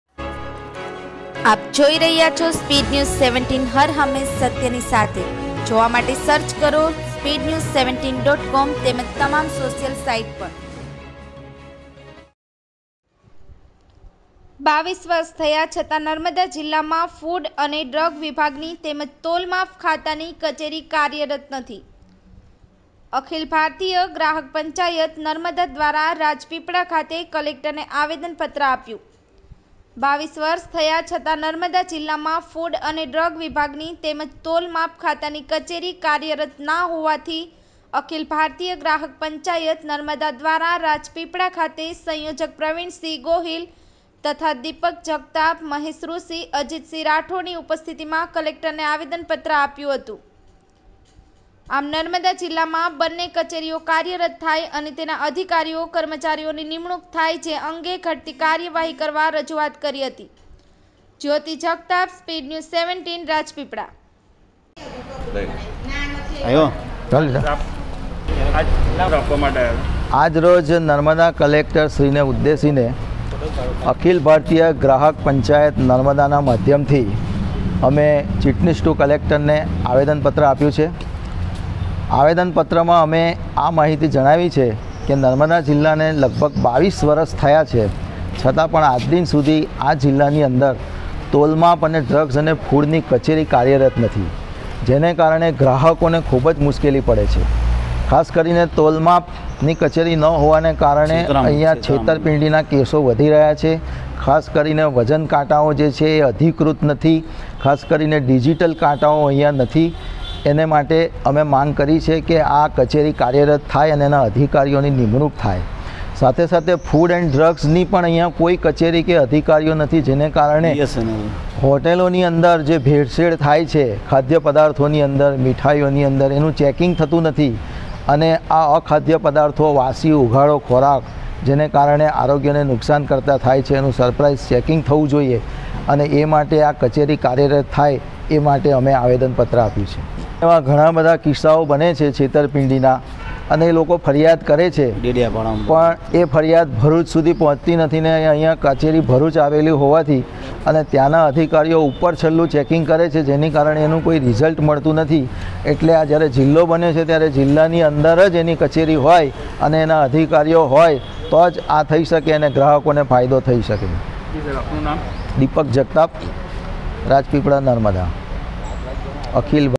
SPEEDNEWS17 22વર્ષ થયા છતા નર્મદા જિલ્લામાં ફૂડ અને ડ્રગ વિભાગની તેમજ તોલમાપ ખાતાની કચેરી કાર્યરત !
22વર્ષ થયા છતા નર્મદા જિલ્લામાં ફૂડ અને ડ્રગ વિભાગની તેમજ તોલમાપ ખાતાની કચેરી કાર્યરત !નથી!

અખિલ ભારતીય ગ્રાહક પંચાયત,
નર્મદા દ્વારા રાજપીપળા ખાતે કલેકટર ને આવેદન આપ્યુ

22વર્ષ થયા છતા નર્મદા જિલ્લામાં ફૂડ અને ડ્રગ વિભાગની તેમજ તોલમાપ ખાતાની કચેરી કાર્યરત ના હોવાથીઅખિલ ભારતીય ગ્રાહક પંચાયત,નર્મદા દ્વારા રાજપીપળા ખાતેસંયોજક પ્રવીણ સિંહ ગોહિલ, તથા દીપક જગતાપ ,મહેશ રૂષિ, અજિત સિંહ રાઠોડ, ની ઉપસ્થિતિ મા કલેકટર ને આવેદનપત્ર આપ્યુહતું

આવેદન મા જણાવ્યુ હતું કે
 નર્મદા જિલ્લો  બન્યાને લગભગ 22 વર્ષ થયા છે.જિલ્લામા ક્રમશઃ સુંદર વિકાસ પણ થઈ રહયો છે.પરંતુ હજુ સુધી નર્મદા જિલ્લામા ફૂડ અને ડ્રગ વિભાગની કચેરી તેમજ તોલમાપ ખાતાની કચેરી  કાર્યરત થઈ નથી!

નર્મદામા ફૂડ અને ડ્રગસ વિભાગની કચેરી ન હોવાથી જિલ્લા મા હોટેલ, રેસ્ટોરાં તેમજ ખાણીપીણીની લારીઓ મા મીઠાઈ તેમજ અન્ય ખાધ્ય પદાર્થોમા ભેળસેળ થતી હોય છે.ઓછા ભાવે હલકી ગુણવત્તાની મીઠાઈ તેમજ અન્ય ખાધ્ય પદાર્થોમા પણ ભેળસેળ થાયતો તેની ચકાસણી સમય સમય પર અને આકસ્મિક ચેકિંગ થતું નથી.! એ ઉપરાંત હોટેલોમાં  વપરાતા પાણીમા ક્લોરીનેશન કરાતું નથીહોટેલોમાં ની બહાર ક્યા તેલનો વપરાશ કર્યો છે તેનુ બોર્ડ મરાતુ નથી! હોટેલો રેસ્ટોરાંમા ગુણવત્તા યુક્ત ખાદ્ય પદાર્થો વપરાય છે કે નહી તેનુ પણ ચેકિંગ થતુનથી! તેથી કેટલાક લેભાગુ વેપારીઓ તેનો ગેરફાયદો ઉઠાવે છે. હોટેલોમાં ઉઘાડો અને વાસી ખોરાક વેચાતો હોય તો આરોગ્ય માટે નુકસાન કર્તા છે. નર્મદા જિલ્લામાં આ કચેરી કાર્યરત ન હોવાથી અને આ કચેરી ભરૂચ જિલ્લામાં આવેલી હોવાથી માત્ર તહેવારો ટાણે ફૂડ ઇન્સ્પેક્ટરો ચેકિંગ કરવા આવતા હોય છે.જે માત્ર ઔપચારિક અને ઉપરછલ્લું ચેકિંગ થતું હોય છે. દર વર્ષે એકાદવાર ચેકિંગ કરવા આવે છે અને સેમ્પલ પણ લે છે .આજ દિન સુધી કોઈ ભેળસેળ પકડાઈ નથી! અર્થાત અહીંયા માત્ર ઉપરછલ્લું જ ચેકિંગ થાય છે .જેને કારણે ગ્રાહકોને નુકસાન અને અન્યાય પણ થાય છે. ઉપરાંત નર્મદા જિલ્લામાં તોલમાપની કચેરી ઘણા .વર્ષોથી કાર્યરત નથી! જેના કારણે નાના મોટા વેપારીઓ તોલમાપમાં છેતરપિંડી કરી ગ્રાહકોને છે કરતા હોય છે .ખાસ કરીને શાકભાજી અને અનાજ વેચવા માટેના વજનીયા, કાટલા અધિકૃત હોતા નથી. મોટાભાગના વેપારીઓ ડિજિટલ વજન કાંટો વાપરતાં નથી.ખાસ કરીને છૂટક લારીવાળા પસ્તીનું અને ભંગારનું વેચાણ કરતી વખતે સ્પ્રિંગવાળા બિન અધિકૃત વજન કાંટા વાપરતા હોય છે. જેને કારણે ગ્રાહકને ઓછું વજન આપી આર્થિક નુકસાન કરે છે. ત્યારે ડિજિટલ વજન કાંટા અંગેનું પણ સઘન ચેકિંગ થવુ જરૂરી છે.
એ જ પ્રમાણે તેલ ,કેરોસીન કે અન્ય પ્રવાહીના વેચાણ માટેનાં માપલા પણ અધિકૃત હોતા નથી! જેને કારણે ગ્રાહકનું શોષણ થાય છે. અને નુકસાન પણ થાય છે
ત્યારે નર્મદા જિલ્લામાં તોલમાપ ની કચેરી પણ હોવી આવશ્યક છે.

આમ નર્મદા જિલ્લામાં બંને કચેરીઓ કાર્યરત થાય અને તેના અધિકારીઓ, કર્મચારીઓની નિમણૂક થાય .જે અંગે ઘટતી કાર્યવાહી કરવા રજૂઆત કરી હતી

રિપોર્ટ: જયોતિ  જગતાપ,રાજપીપળા